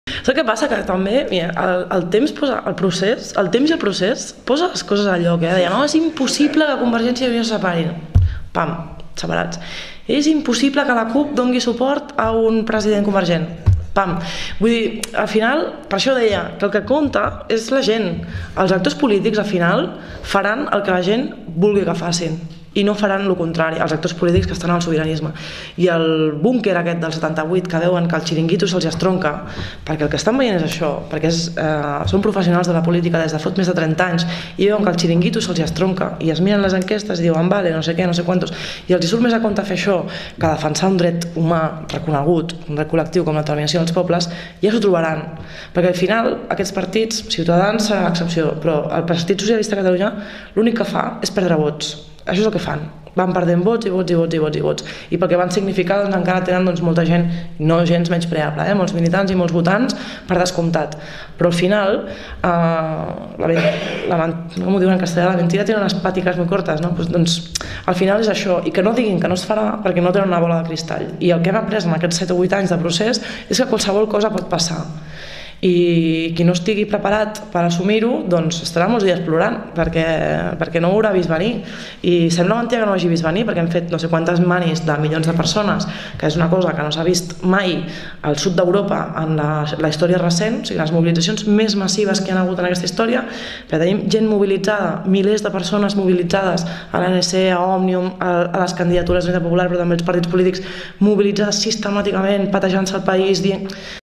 El búnquer del 78 veu que el «xiringuito» se’ls estronca
Conseqüències pels partidaris del Regne d’Espanya d’haver negat fins al darrer moment fets ben previsibles.
Extracte de la xerrada de Núria Gibert, portaveu del Secretariat Nacional de la CUP, sobre el Referèndum que s’ha realitzat a Riudoms.